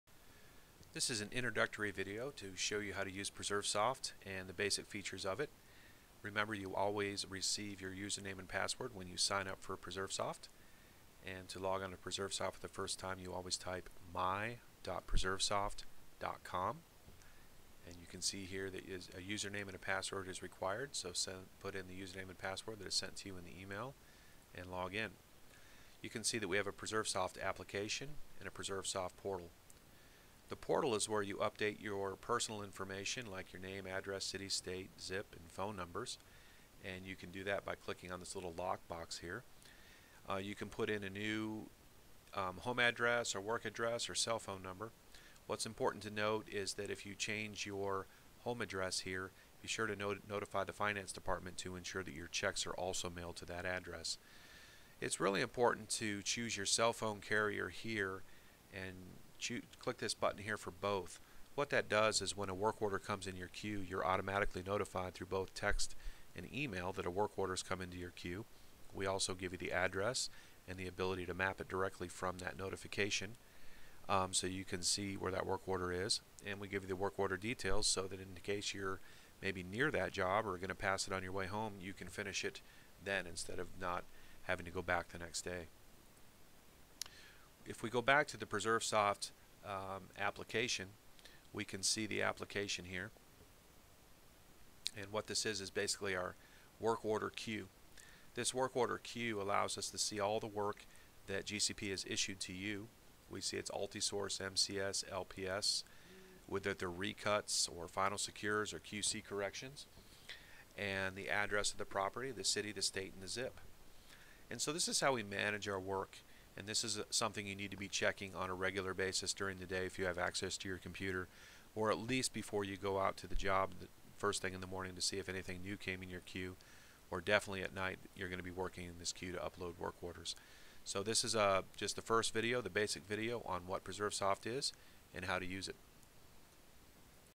PreserveSoft Video 1
Using PreserveSoft (1).  Using the Portal and Basics of the Work Order Queue.